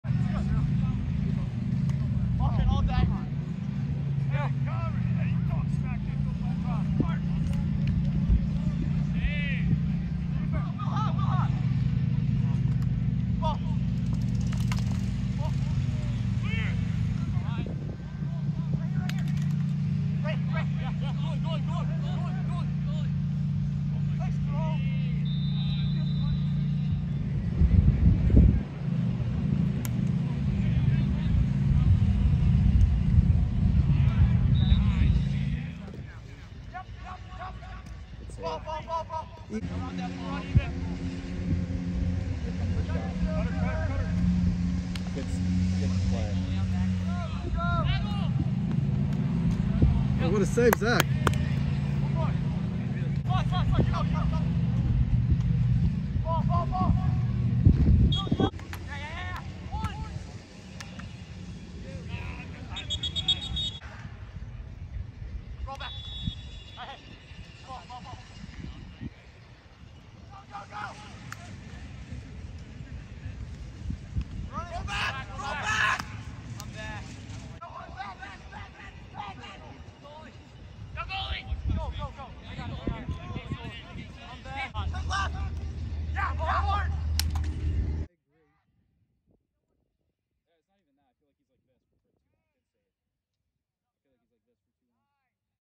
Zach Rosenberg Goalie 91MD25 & STA 25 @ Lax Masters 7.25.23 Team A Pod A -Michigan. Princeton & Duke
Listen with Volume on to hear Goalie Directing the D

Zach Rosenberg 2025 Goalie Team 91 MD 2025
St. Thomas Aquinas High School 2025

Team 91 MD 2025 Goalie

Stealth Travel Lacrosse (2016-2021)
Coach Director: Ryan Burton

UnderArmor/New Balance All American Team South (Summer 2022 & 2023)
Coach Ira Vanterpool

Nike National Team Florida  (Summer 2023)
Coach Montes